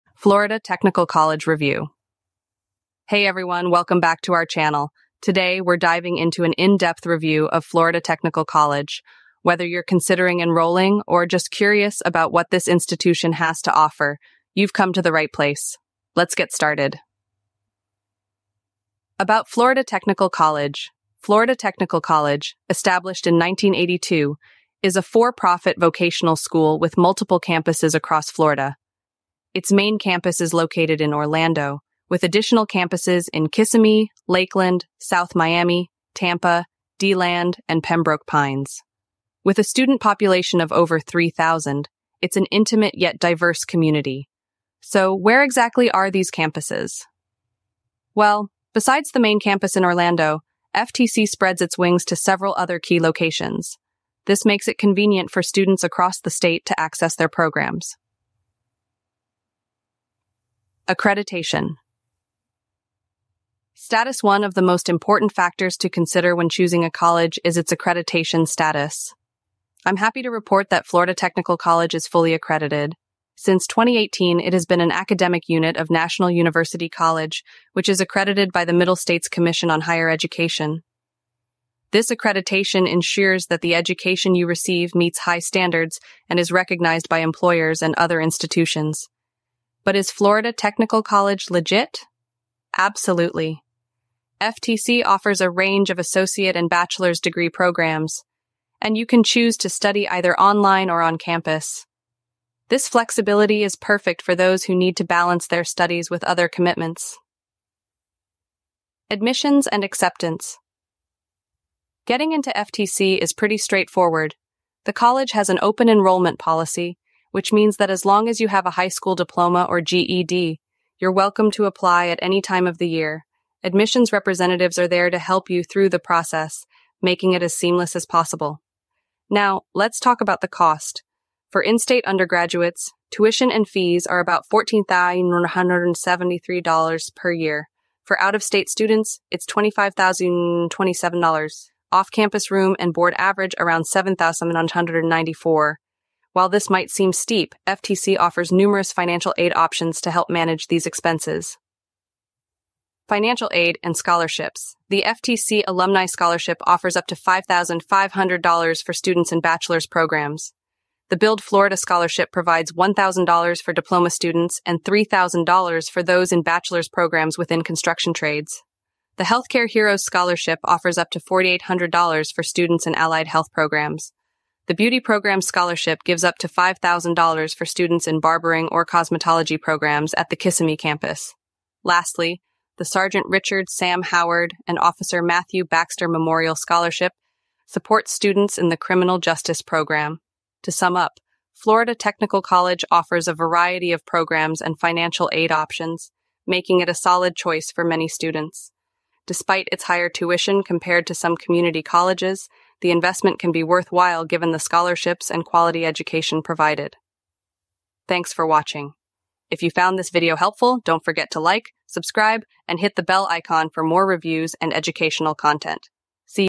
Florida Technical College Review
Are you looking for Florida Technical College Review? You've come to the right place. A student must consider many factors before choosing an academic program at a community college across the country. This article provides an overview of Florida Technical College, along with information about costs, rankings, and popular bachelor's and associate's degree programs.

We have also discussed Florida Technical College's accreditation status in this blog.

The following elements are important to consider when determining whether a school is a good fit for a student's individual needs and future professional goals. In this comprehensive guide, we will highlight a few of these crucial elements.